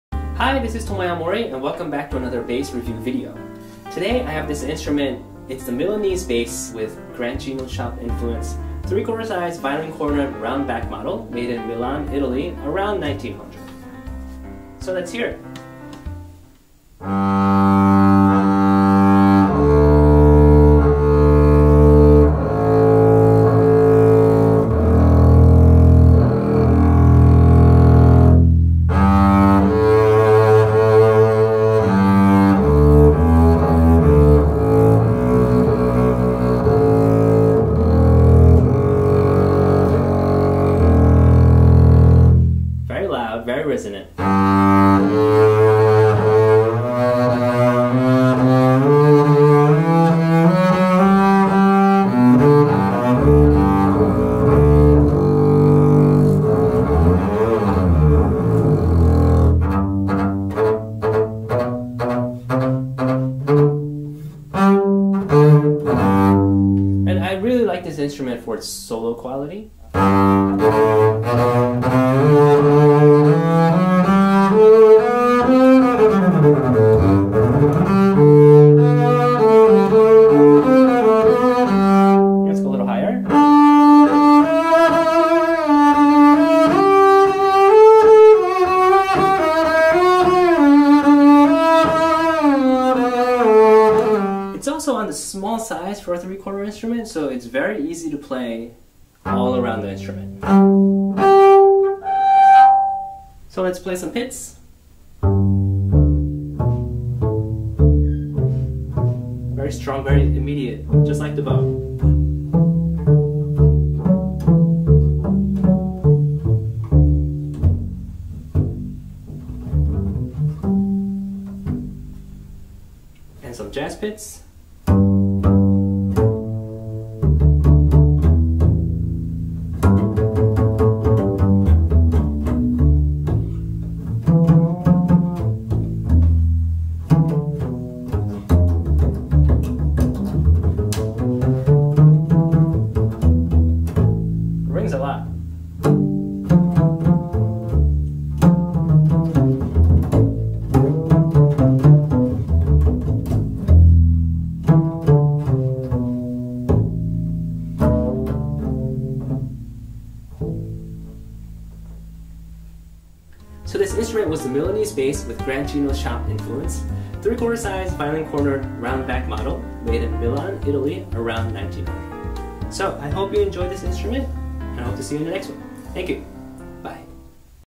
[Kolstein Bass Review] Evaluation of Attributed Milanese Bass with Grancino Shop Influence
Tomoya Aomori's Playing Evaluation of the Attributed Milanese Bass Violin in the Kolstein Collection

Samuel Kolstein & Son, Ltd. Violinmakers

Product Description

Milanese Bass Violin with Grancino Shop influence
3/4 Size, Violin Cornered, Round Back Model
Made in Milan, Italy c. turn of the 1900s

Top table is a two-piece plate of fine grained quarter-cut fir
Back table is a two-piece plate of slab-cut pearwood
Ribs of matching pearwood to that of the back table
Original scroll of pearwood with a slightly figured maple neck graft
Italian style Montavani tuning gears
Varnish is deep reddish brown
Fully restored to excellent condition
Overall body length: 40 3/4 in.
Width at the upper bout: 18 1/4 in.
Width at the center bout: 14 1/4 in.
Width at the lower bout: 22 1/4 in.
Mensure (string length): 40 3/4 in.